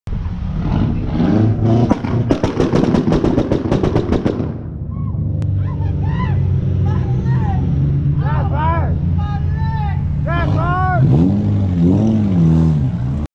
Pop tune with loud exhaust on Volkswagen!
Volkswagen with a pop tune at a stoplight revving up and making some popcorn!